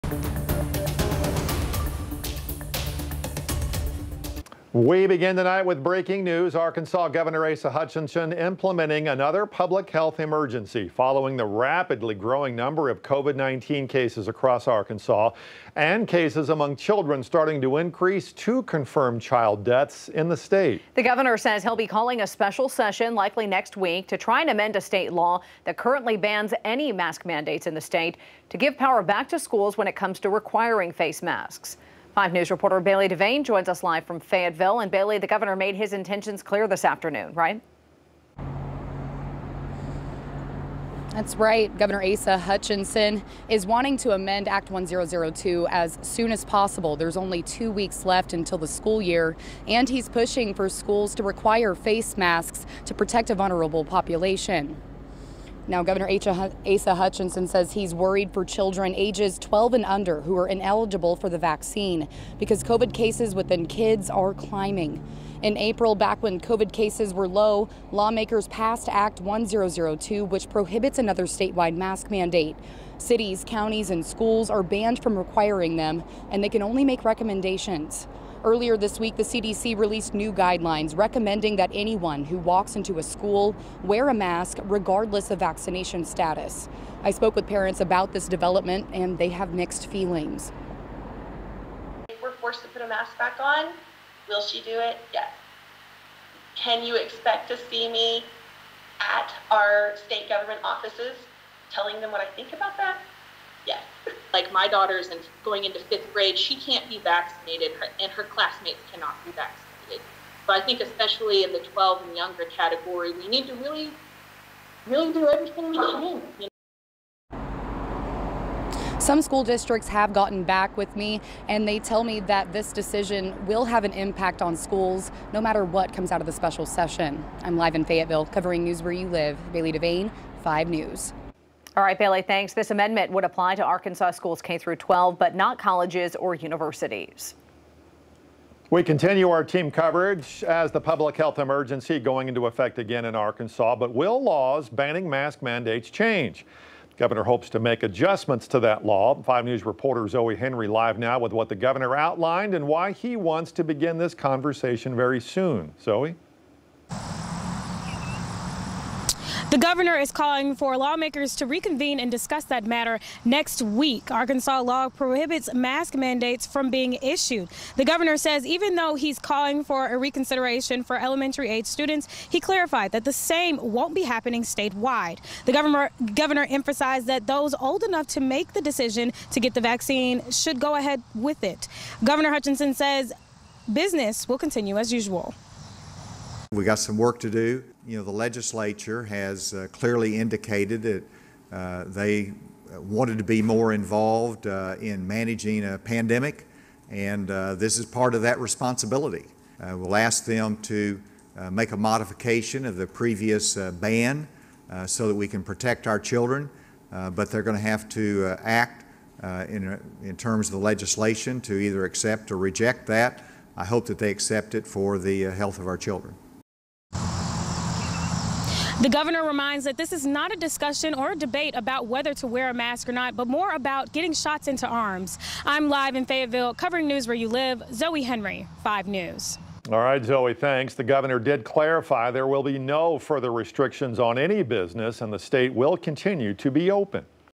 Public health emergency declared in Arkansas, governor pushing to let schools enforce mask mandates
Arkansas is now under another public health emergency as COVID-19 cases surge due to the delta variant.